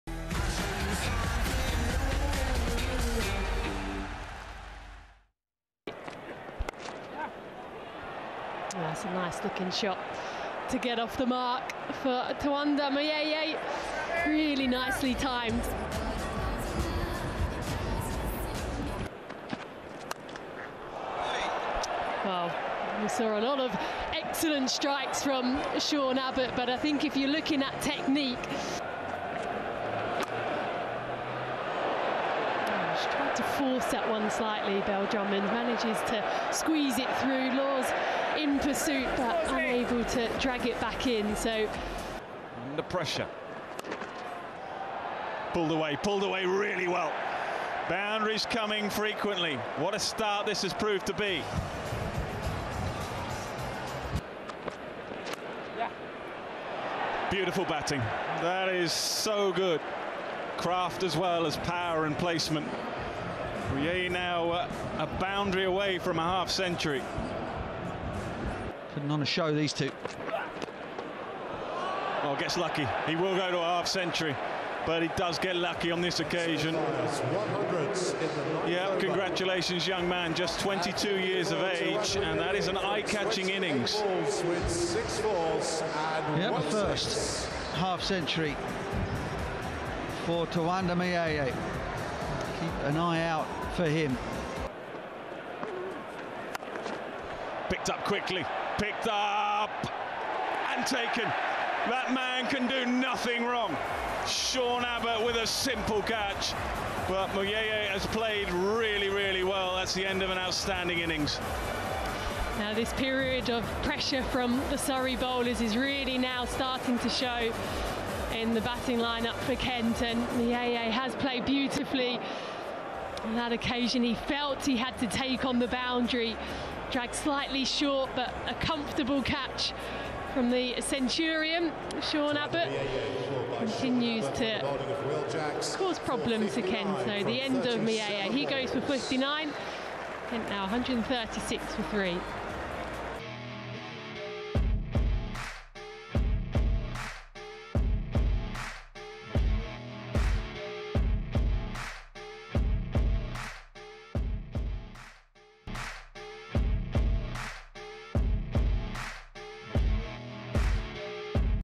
Tawanda Muyeye 59 runs vs Surrey | T20 Blast - SRY vs KEN
🏏: T20 Blast 2023
📺: Willow TV